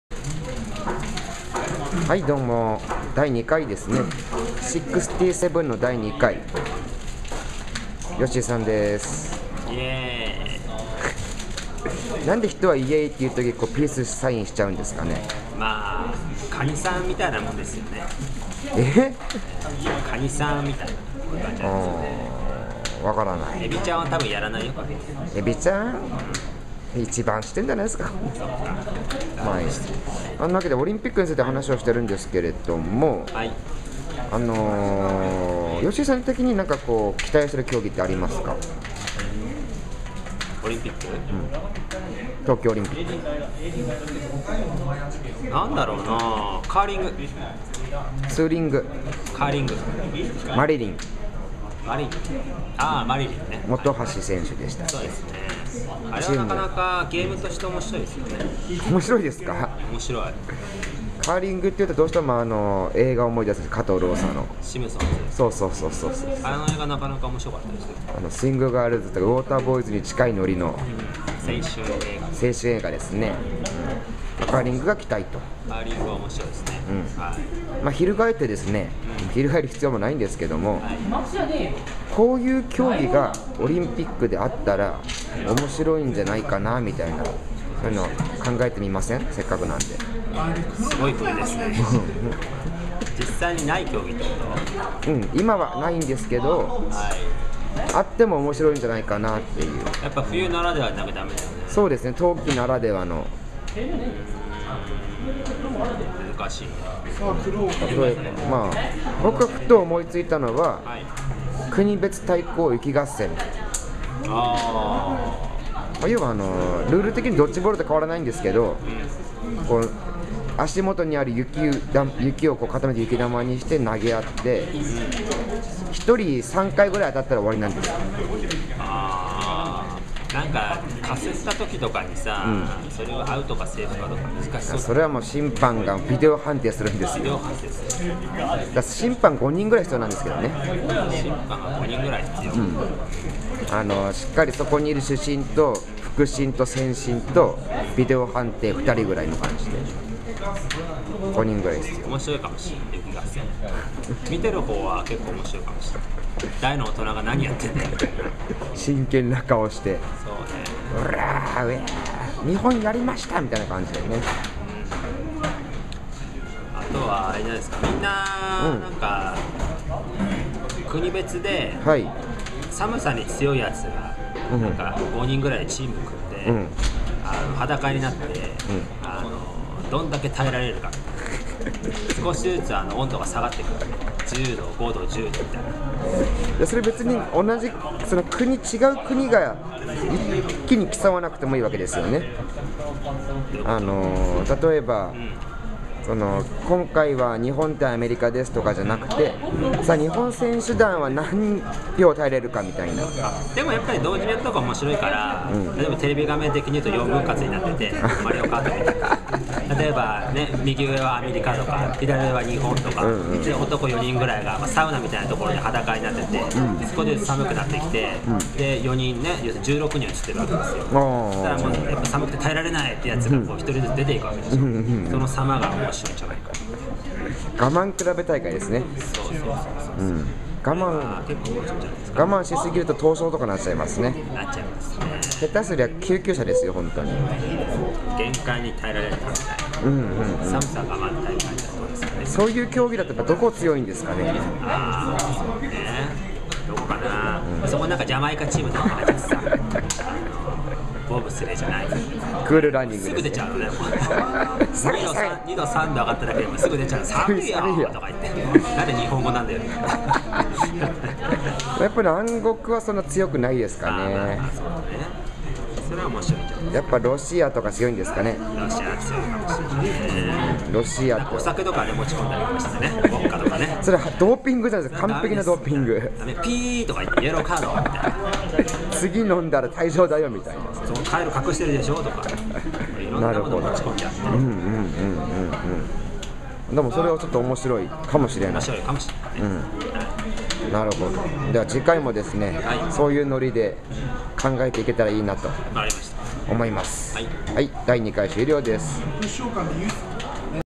冬季五輪の新競技？ ～原宿メモリアル第67回その２
ロウガンラガンズ

カナダのバンクーバーにて
冬季オリンピック真っ只中です。

毎日いろんな競技があります。
原メモも毎日更新です。

今回は冬季オリンピックで
こんな競技があったらどうだろうと、
「新競技」について協議しています。

さぁ、今日はフィギュアスケート女子の
ショートプログラムが行われる日です。

頑張れ！浅田、安藤、鈴木の三選手！！
心の底から応援しています！！